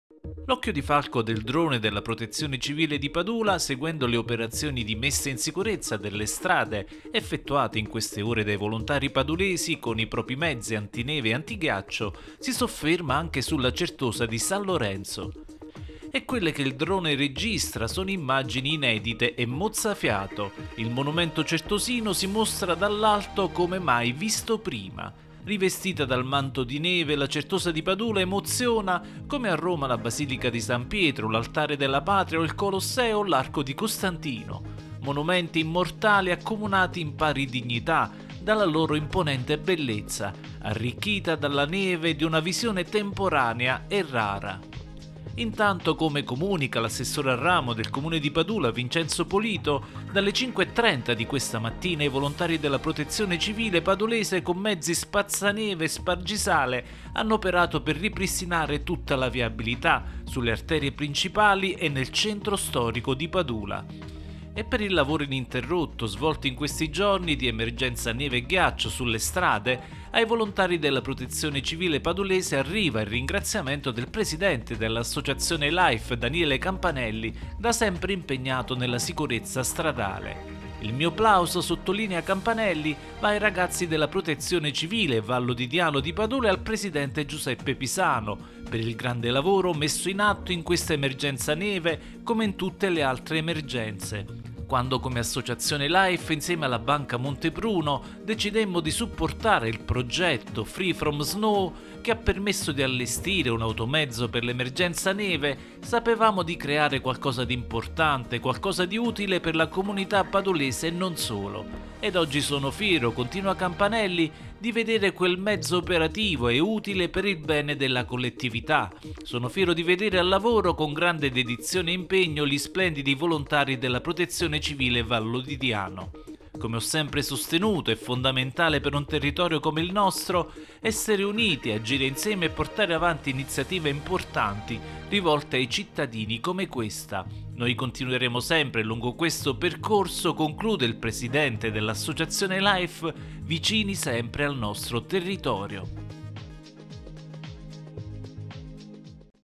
Padula, il drone sulla Certosa innevata. E dall’Ass. Life plauso alla Protezione Civile
Padula, il drone sulla Certosa innevata: immagini mai viste. E dall’Associazione Life plauso alla Protezione Civile. 01/03/2018